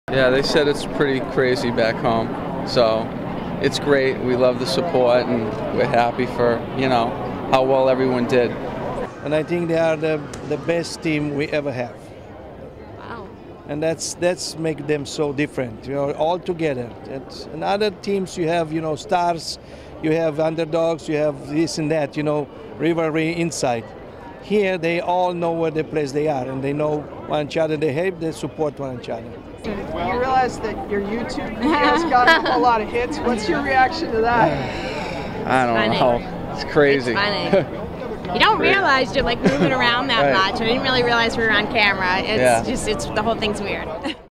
Aly Raisman's Parents React
Needham gymnast Aly Raisman closes the Olympic Games with three medals, including an individual gold she won Tuesday night in the floor event exercise. The win was what
one analyst called a "reversal of fortune"  after missing a medal in individual all-around, the event which made a gold medal winner and celebrity of teammate Gabby Douglas her parents are as stoic as their daughter was when she missed out on a medal in the individual all-around gymnastics final during week one of the Olympics, Rick and Lynn Raisman were interviewed in front of NBC's Today show where the gold medal winner,
Gabby Douglas, was waiting to be interviewed. The Raismans
and the U.S. team's coach instead talked about the performance of fellow Olympians after the team won the gold medal during week one of the Olympics.